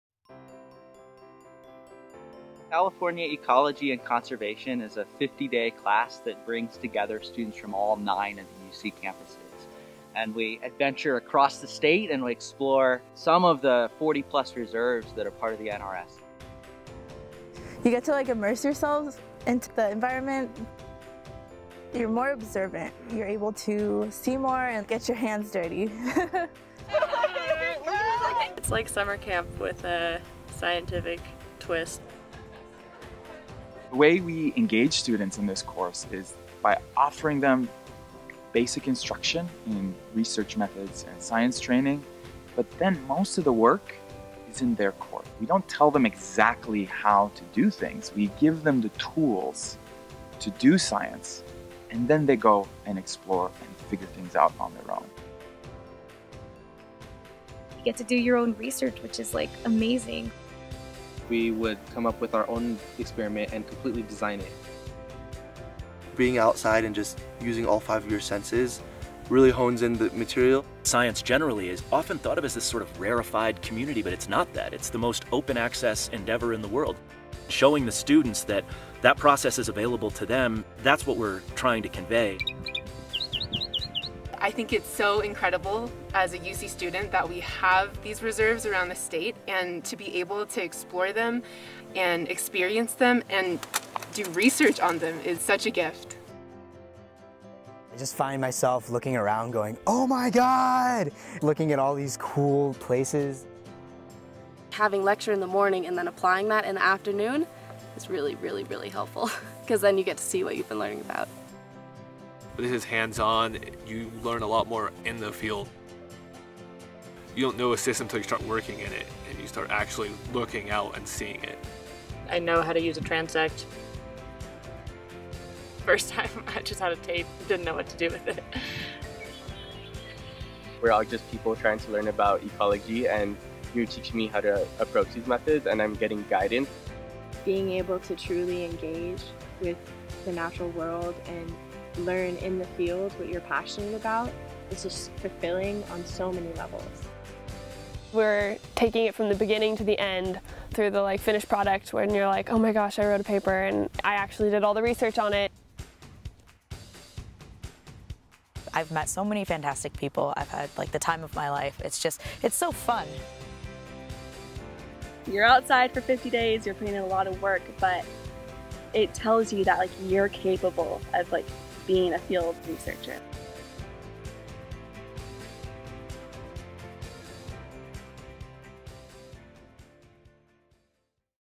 California Ecology and Conservation.August 2019
California Ecology and Conservation brings together 27 students from across the UC system for seven weeks of intensive learning at NRS reserves. Guided by experienced field instructors, undergraduates transform into scientists by conducting independent research studies. Students learn to notice natural patterns, frame questions into feasible research projects, and practice standard techniques such as surveys of animal and plant populations. At the conclusion of each project, students analyze their data and present their findings to the class in oral presentations, posters, and reports. Students hone their research, public speaking, and scientific writing skills with constant practice and feedback. All the while, students gain a working familiarity with California’s diverse ecosystems while immersed in the NRS’s classrooms without walls.